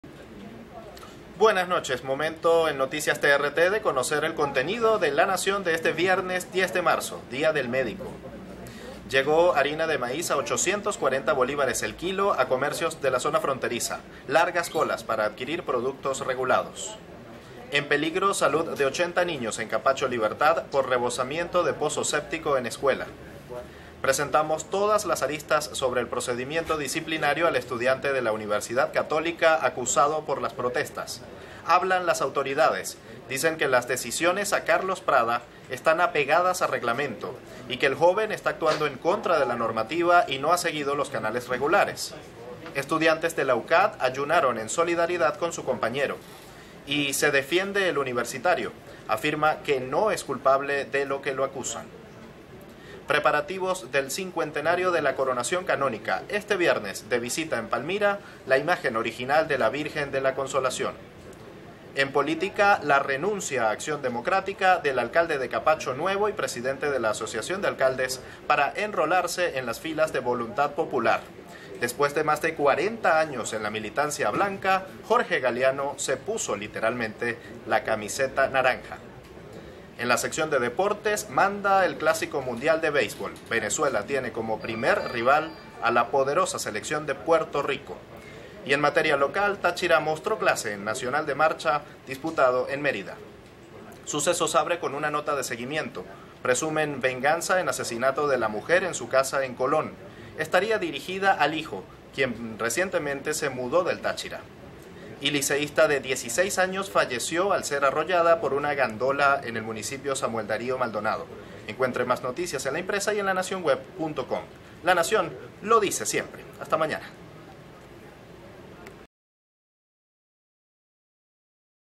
Harina de maíz a 840, arrollada por gandola y otros titulares
Titulares de las noticias que ud podrá leer en la edición del viernes 10 de marzo de 2017